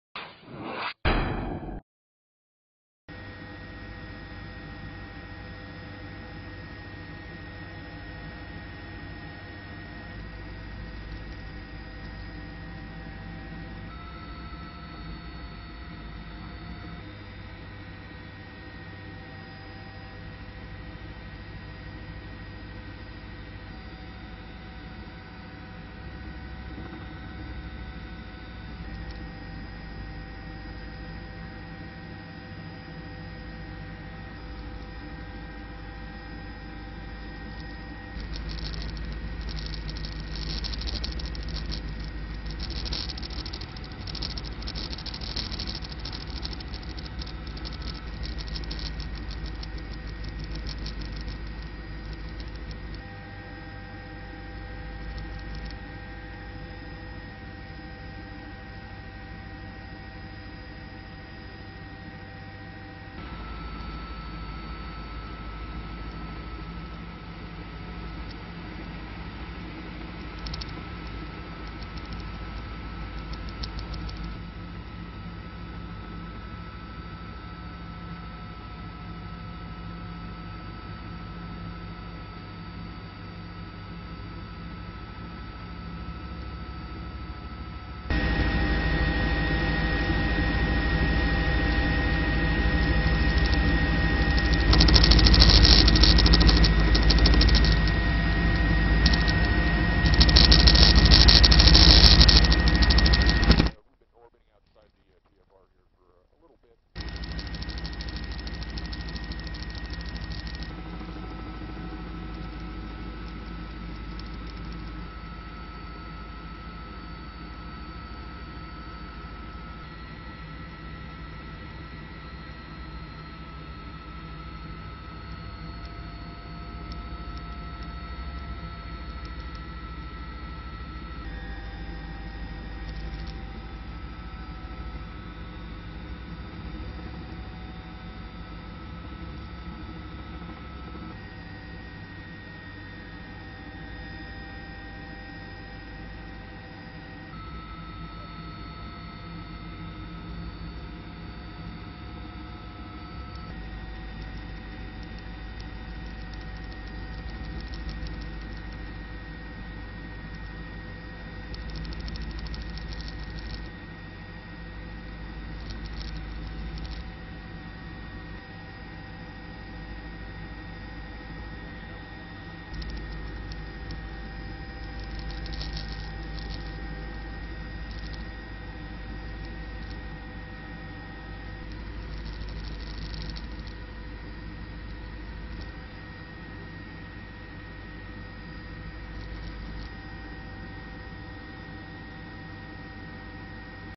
Sky 7 extended video over the Tres Lagunas fire
See extra video from Sky 7's Friday flight above the Tres Lagunas fire burning 10 miles north of Pecos, N.M.